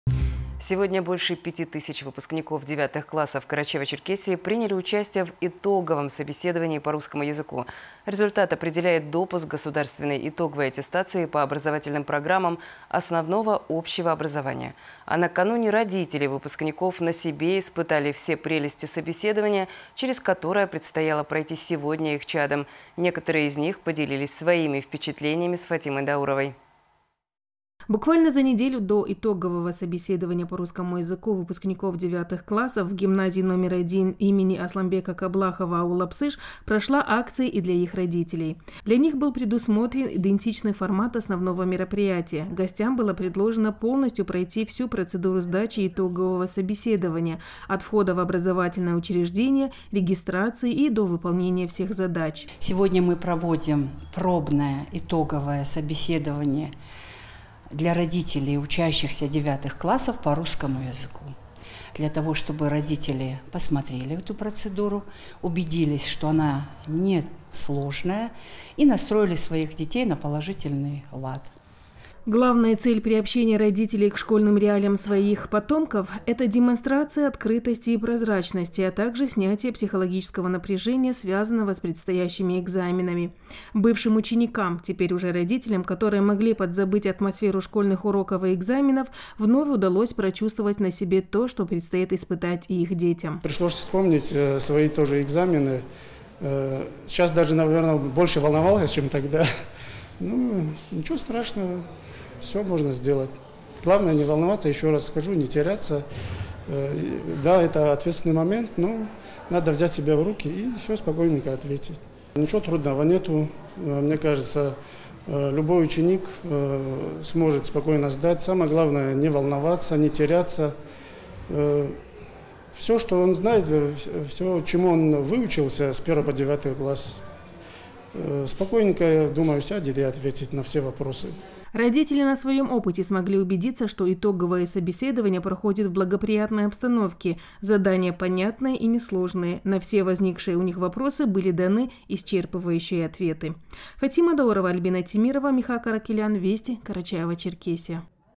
Девятиклассники Карачаево-Черкесии приняли участие в итоговом собеседовании по русскому языку
Больше 5.000 выпускников девятых классов Карачаево-Черкесии приняли участие в итоговом собеседовании по русскому языку. Результат определяет допуск к государственной итоговой аттестации по образовательным программам основного общего образования. А накануне родители выпускников на себе испытали все прелести собеседования, через которые предстояло пройти их чадам